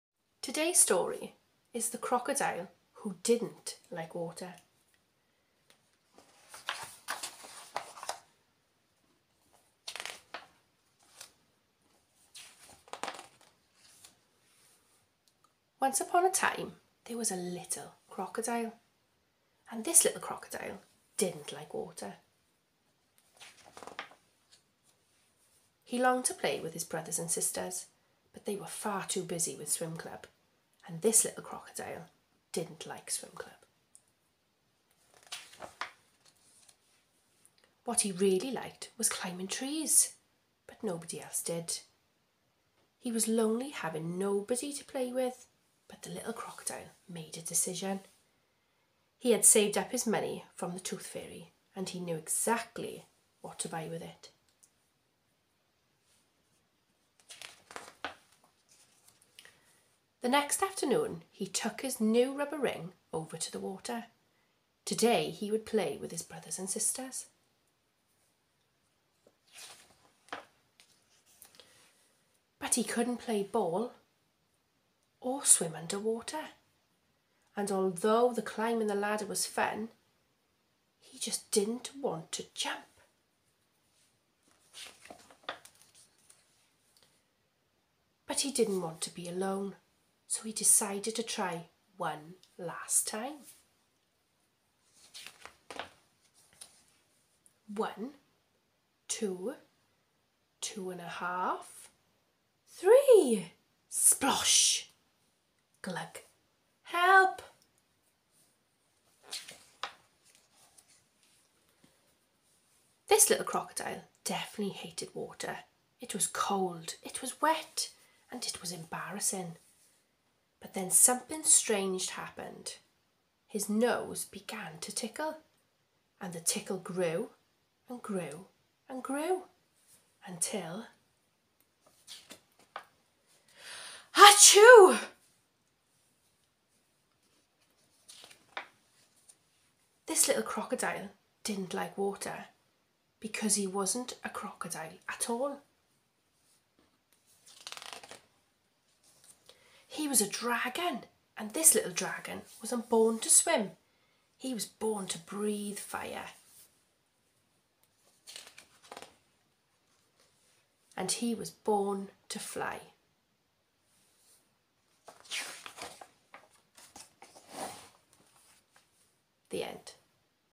The Crocodile who didnt like water
can you find out why the crocodile didn't like water? is he a crocodile?